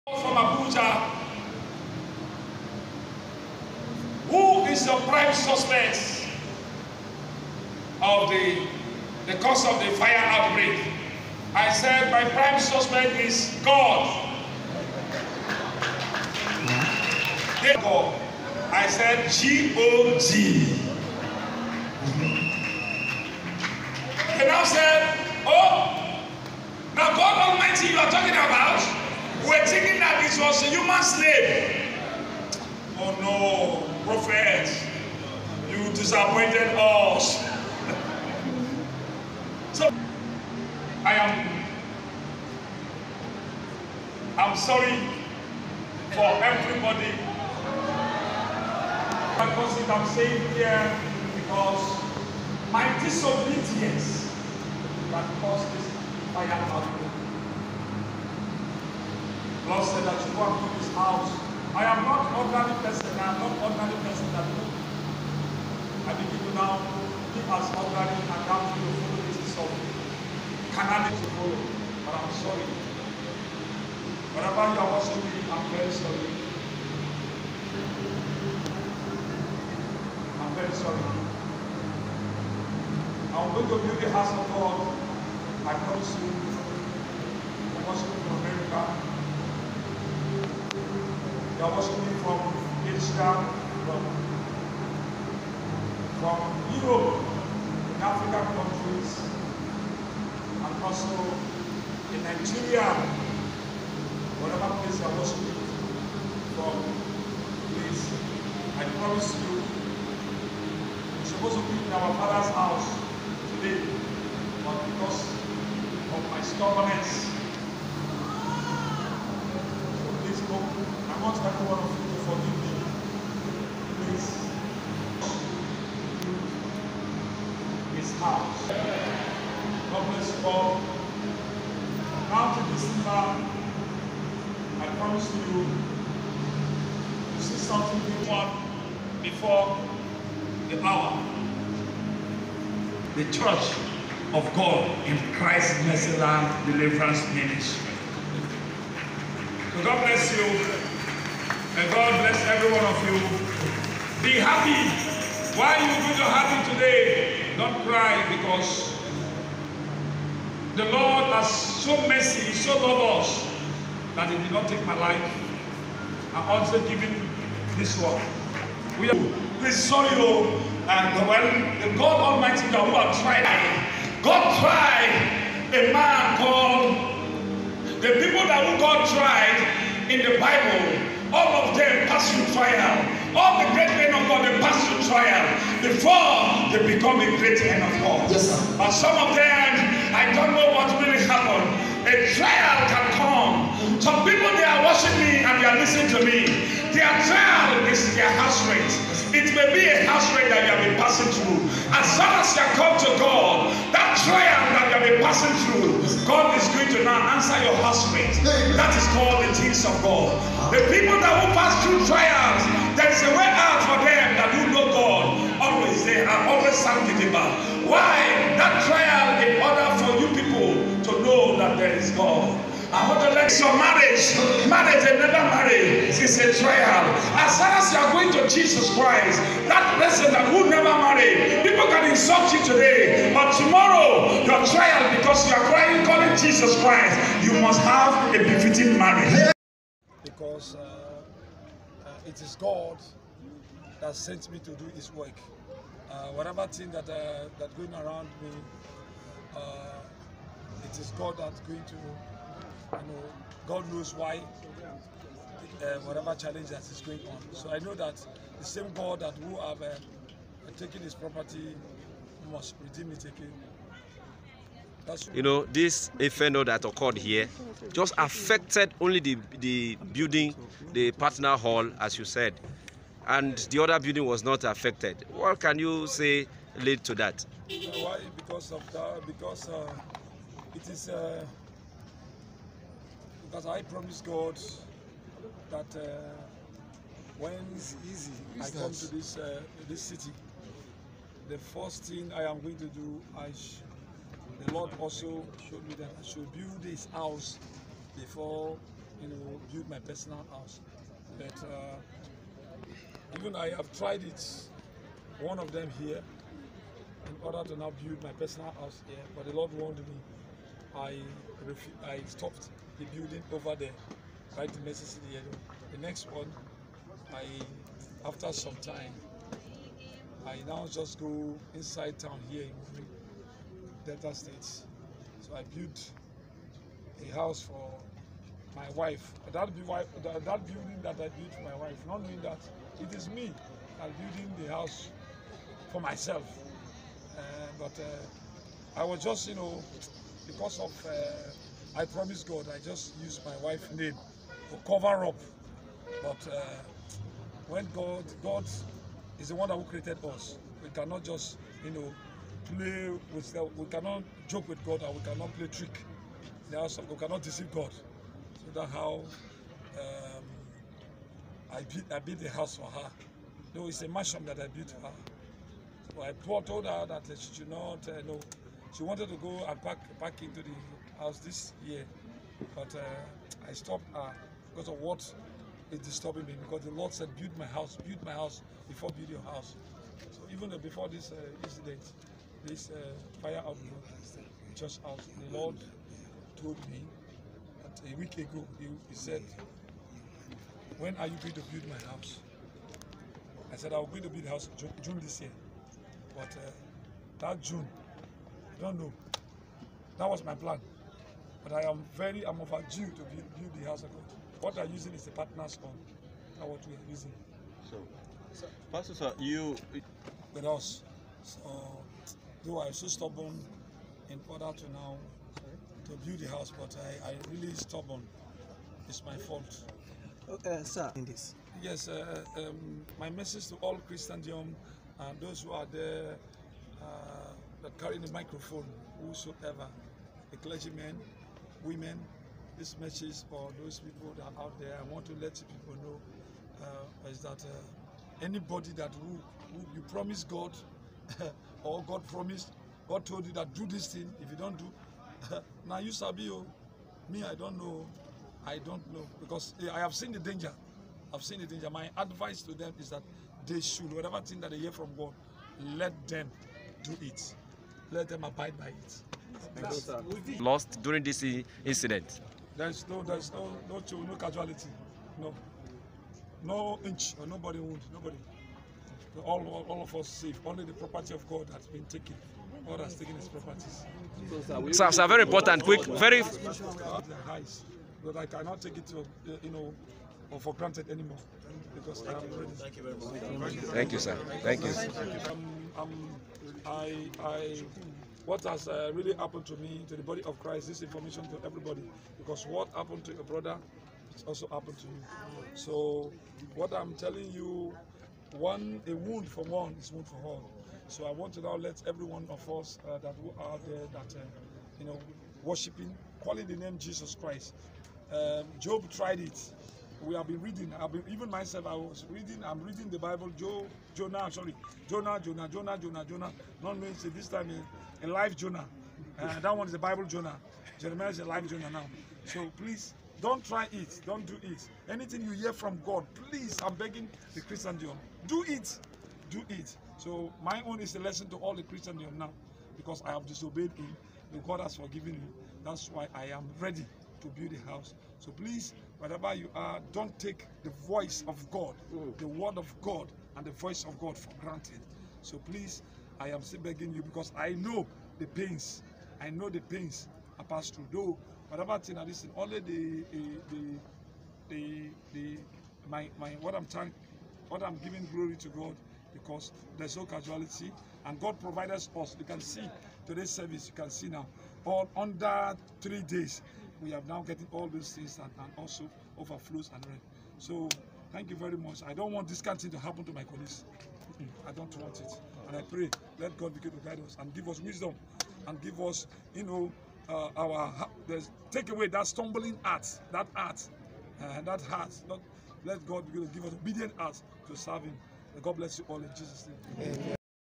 Disobedience to Build Church Auditorium: Billionaire Prophet Fufeyin apologises to God, members
Billionaire Prophet and Founder/ General Overseer of Christ MercyLand Deliverance Ministries, CMDM, Worldwide, Jeremiah Omoto Fufeyin, has openly apologized to God and members of CMDM for his “stubbornness and disobedience to God’s instructions”, for him to build a befitting auditorium for worshipping God.

Our Principal Correspondent reports that Prophet Fufeyin, tendered the apology Sunday March 26, during a live service, urging members of Christ MercyLand Deliverance Ministries, CMDM, Worldwide, not to feel bad, but sustain their faith in God’s word and the ministry.